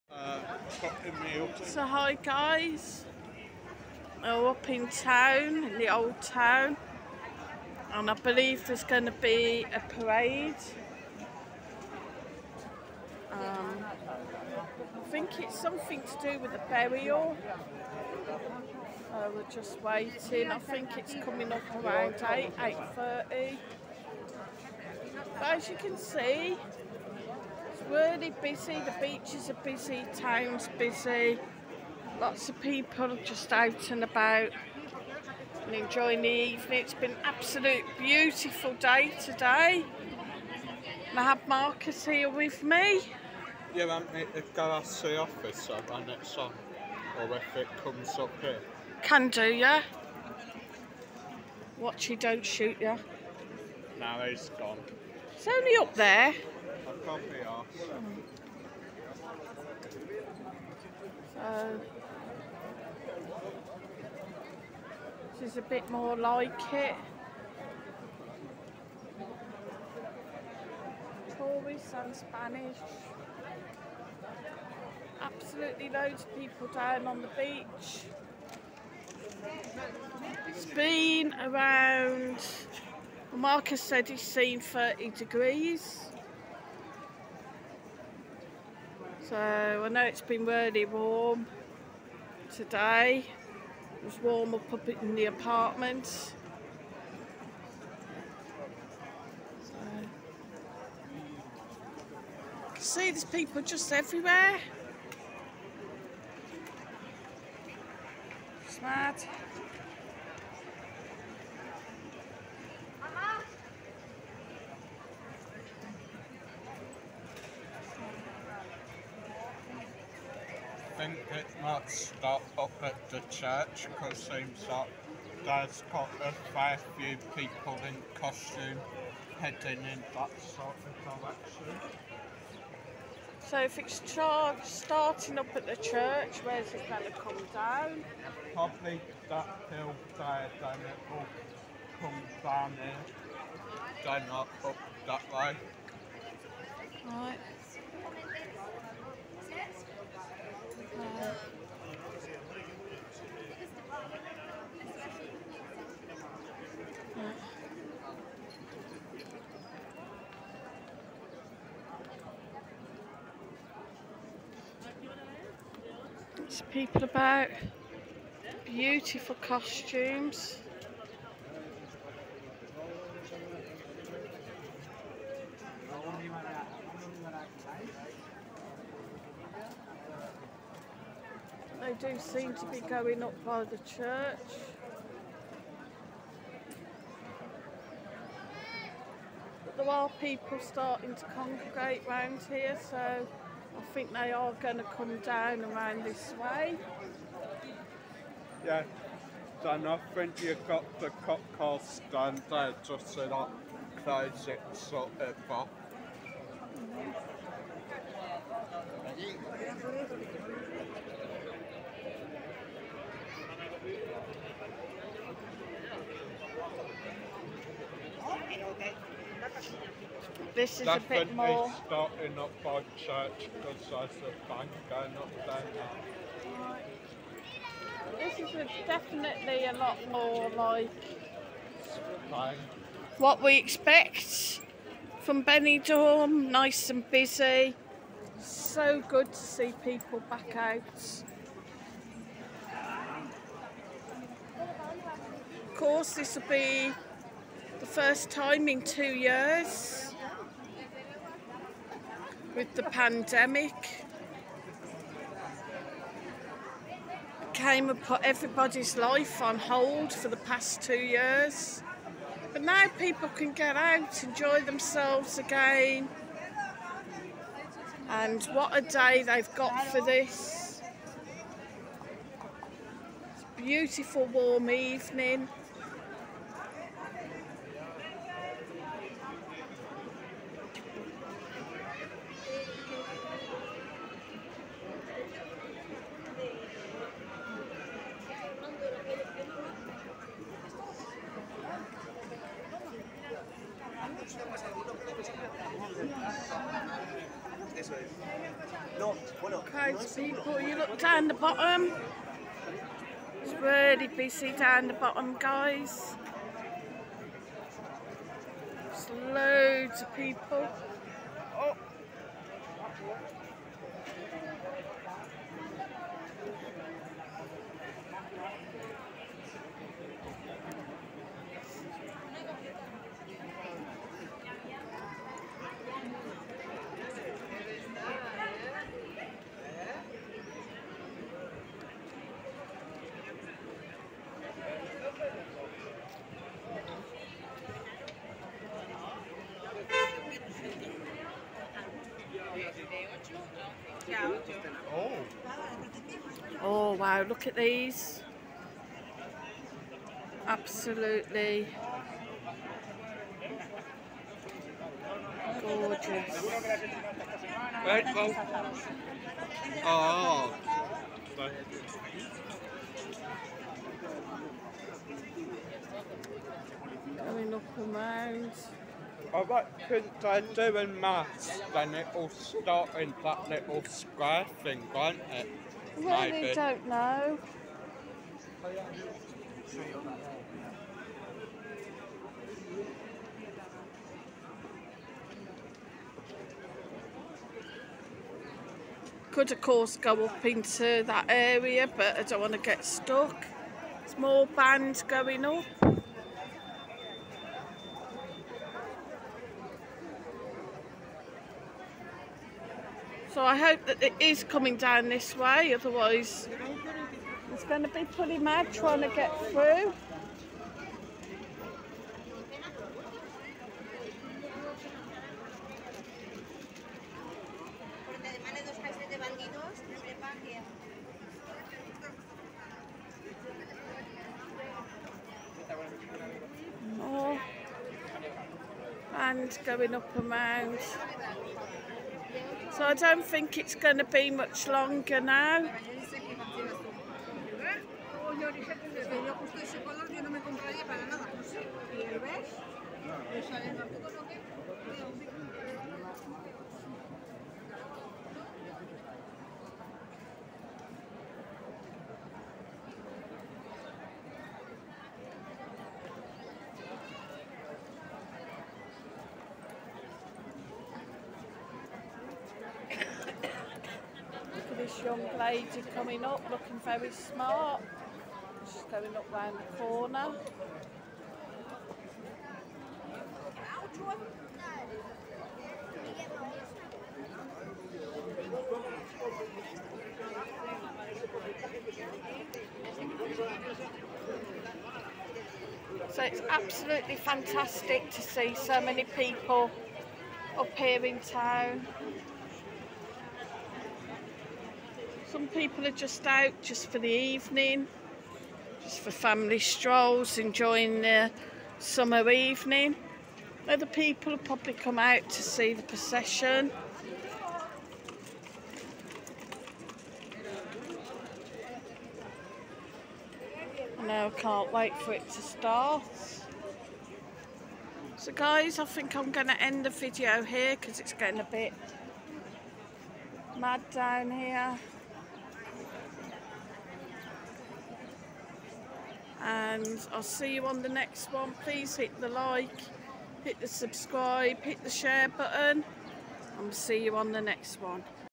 BENIDORM TOWN BUSY
waiting for the Easter parade & town is busy, loads of people, good to see